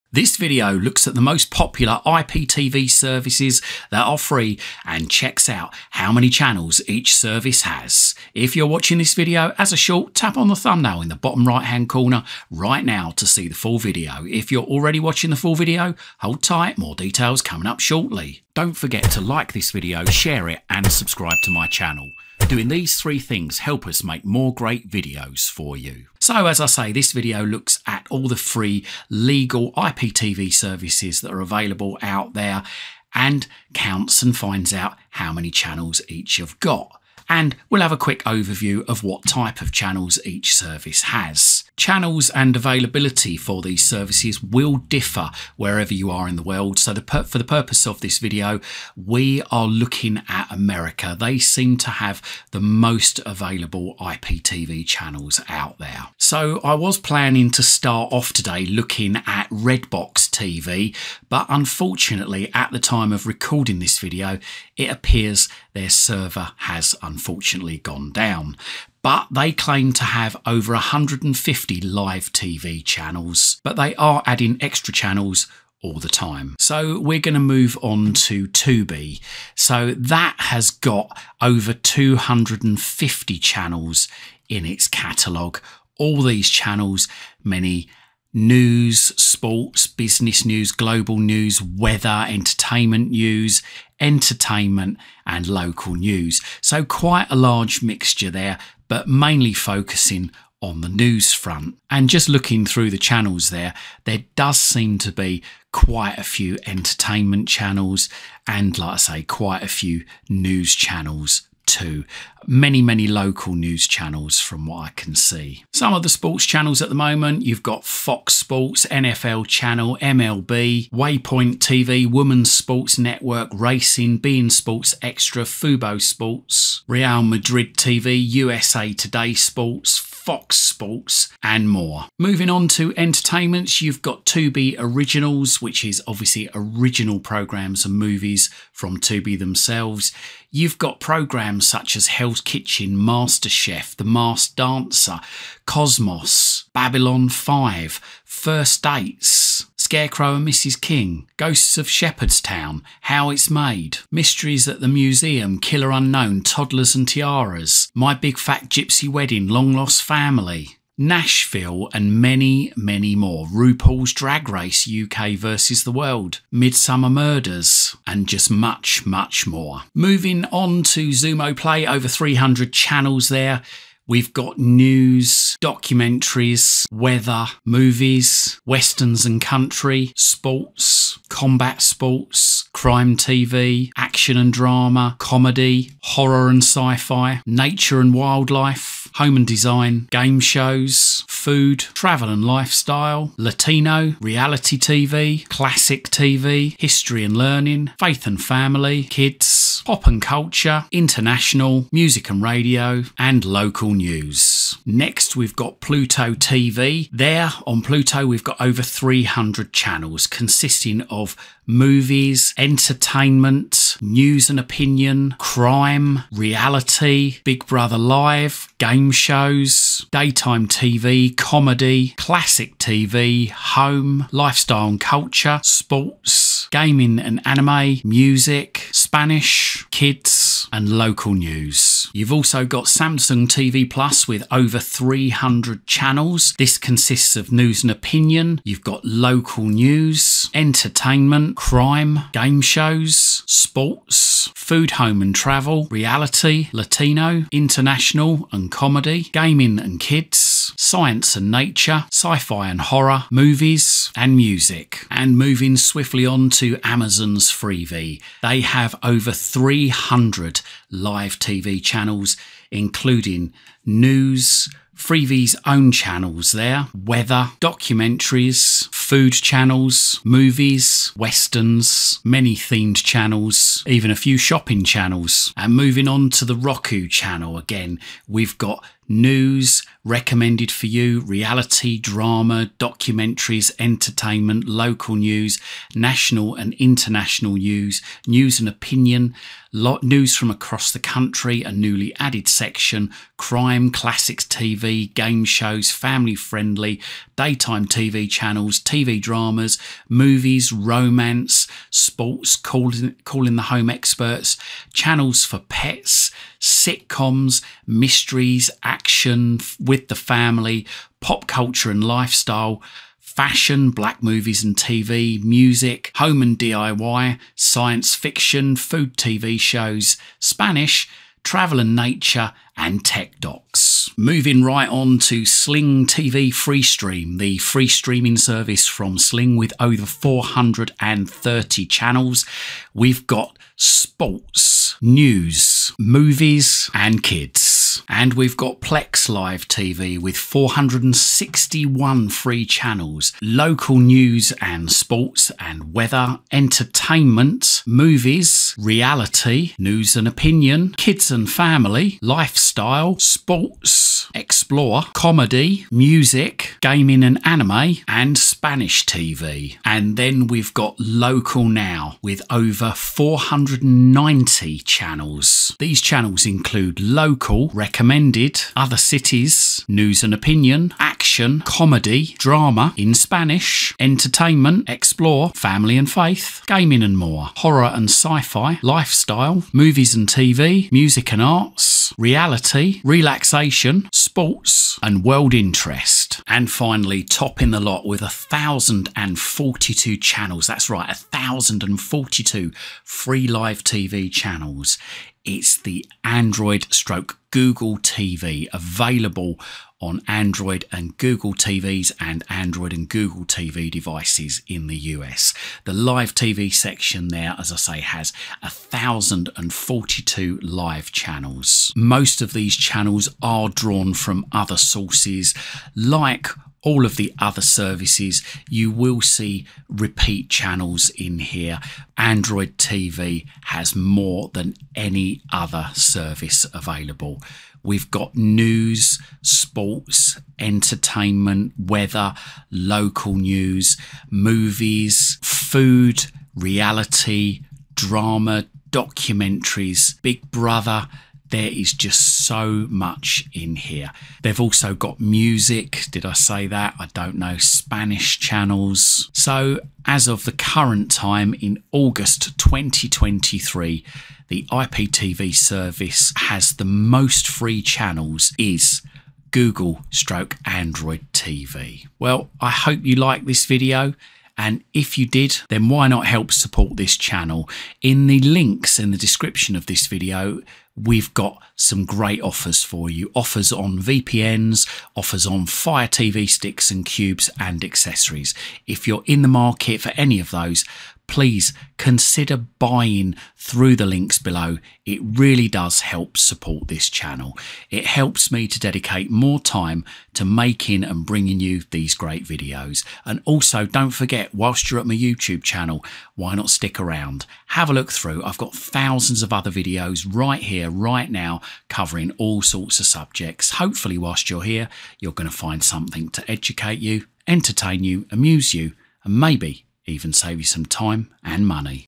✅ Which IPTV Service has the most Channels? ✅
This video looks at the various Legal IPTV Services and looks at how many channels each service has.